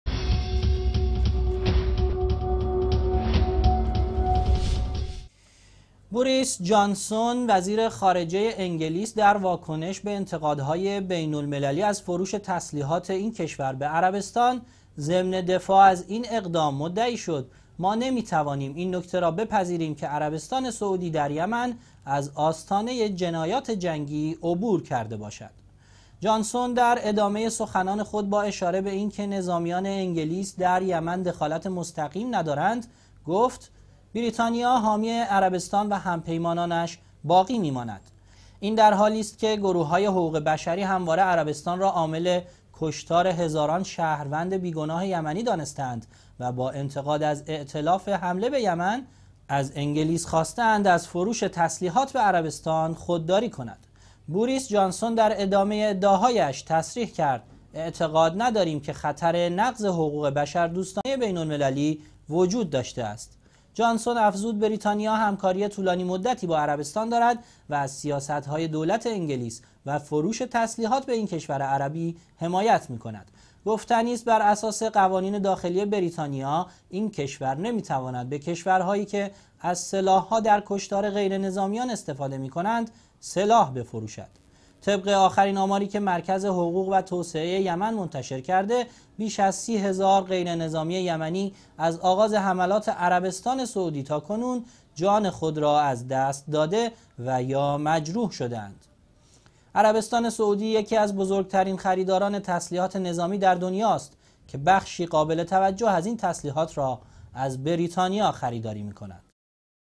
حمایت وزیر خارجه انگلیس از بمباران یمن / ۱۶ آذر ۱۳۹۵
حمایت وزیر خارجه انگلیس از بمباران یمن

«بوریس جانسون» وزیر خارجه انگلیس در واکنش به انتقادهای بین‌المللی از فروش تسلیحات این کشور به عربستان سعودی دفاع نموده و طی ادعایی گفت: ما نمیتوانیم بپذیریم که عربستان سعودی در یمن، از آستانه جنایات جنگی عبور کرده باشد.

جانسون در ادامه سخنان خود با اشاره به این که نظامیان انگلیس در یمن دخالت مستقیم ندارند، گفت: ولی بریتانیا حامی عربستان و همپيمانانش باقی می‌ماند این در حالیست که گروه‌های حقوق بشری همواره عربستان را عامل کشتار هزاران شهروند بیگناه یمنی دانسته و ائتلاف حمله به يمن را مورد انتقاد قرار داده و از انگلیس خواسته‌اند جلوی فروش تسلیحات به عربستان را بگیرد.

وی در ادامه ادعاهایش پیرامون حملات هوایی عربستان در یمن گفت «اعتقاد نداریم تا کنون خطر نقض حقوق بین‌الملل بشردوستانه وجود داشته است.»

جانسون افزود: بریتانیا همکاری طولانی مدتی با عربستان دارد و از سیاست‌های دولت انگلیس و فروش تسلیحات به این کشور عربی حمایت می‌کند.

گفتنی است بر اساس قوانین داخلی بریتانیا، این کشور نمی تواند به کشورهایی سلاح بفروشد که از آن سلاح ها در کشتار غیر نظامیان استفاده می کند.

طبق آخرین آماری که مرکز حقوق و توسعه یمن منتشر کرد، بیش از 30 هزار غیر نظامی یمنی، از آغاز حملات عربستان سعودی تا به امروز جان خود را از دست داده و یا مجروح شده اند.

گفتنی است، عربستان سعودی یکی از بزرگترین خریداران تسلیحات نظامی در دنیاست که مقدار زیادی از این تسلیحات را از بریتانیا خریداری می نماید.

سه شنبه - ۱۶ آذر ۱۳۹۵